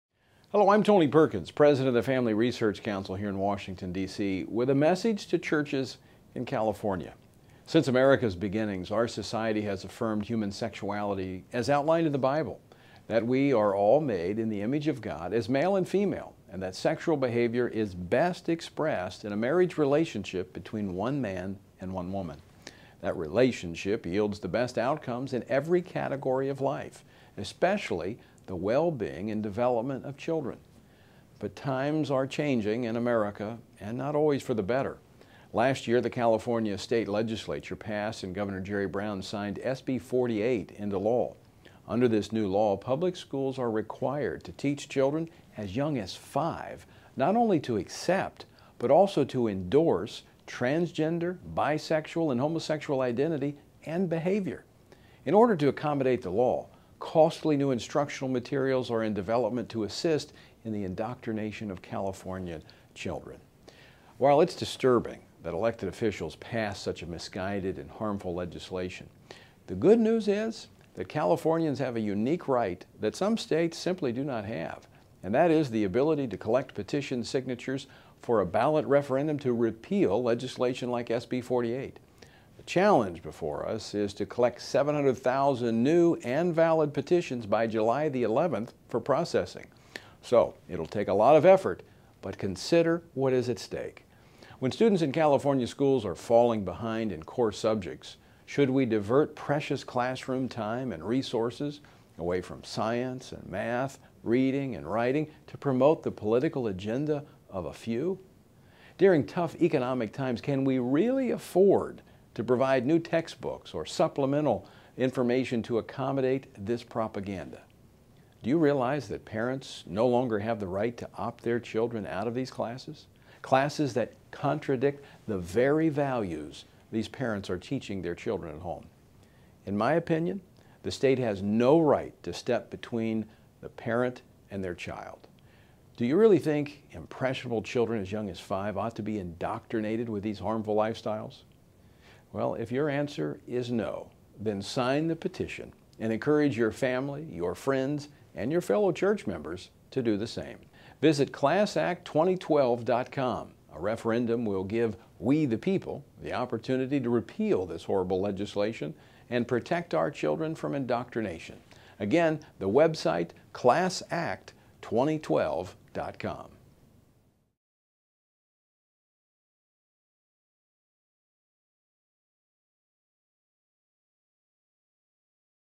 Repeal SB 48
to take action.  California Gov. Jerry Brown signed a new law, Senate Bill (SB) 48, mandating that public school Social Science curriculum and instruction include the role and contributions made to the State of California (and the nation) by individuals who identify as transgender, lesbian, gay, and bisexual. Instruction starts in Kindergarten, and parents who object to the subject matter cannot opt out their children. To sign the petition to repeal SB 48 you must be registered to vote in California. and download, print, sign, and mail the petition to the Sacramento office.